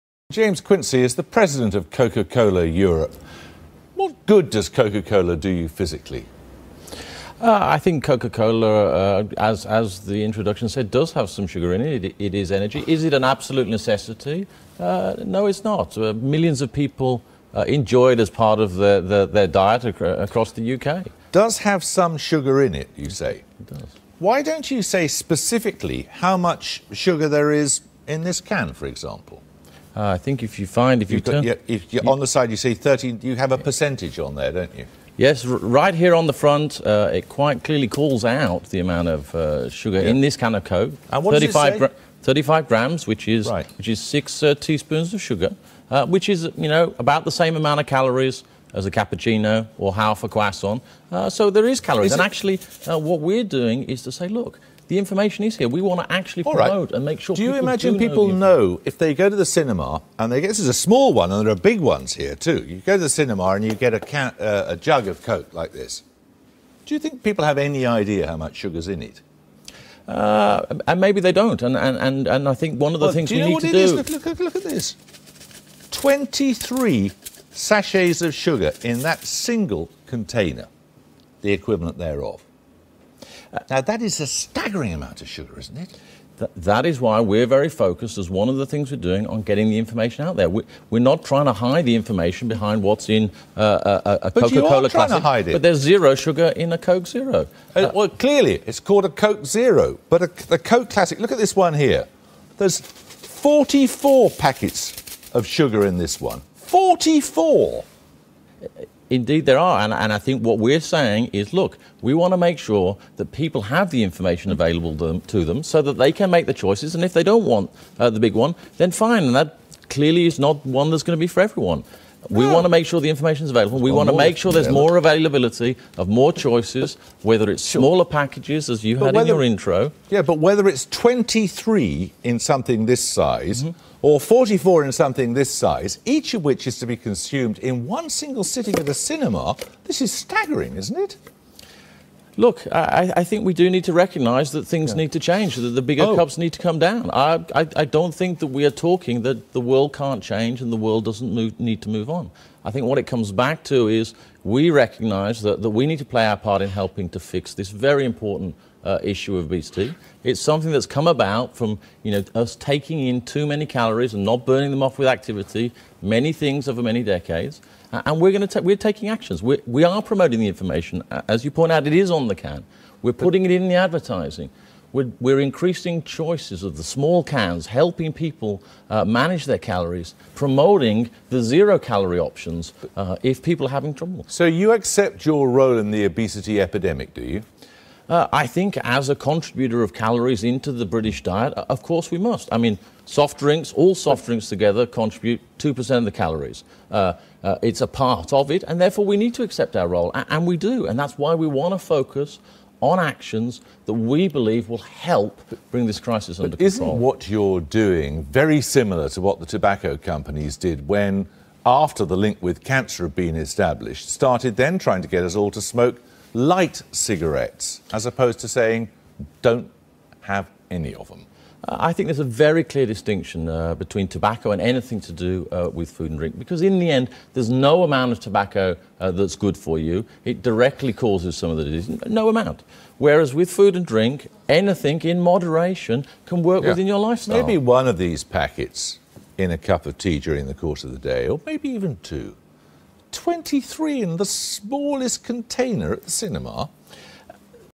Critica: ¿Cuanta azucar hay en una Coca-Cola?
Es un video de la bbc de londres donde enfrentan al presidente de Coca-Cola europa de cuanta azucar tiene una Coca Cola, es muy solprendente ver cuanta azucar tiene una Coca y lo malo que es.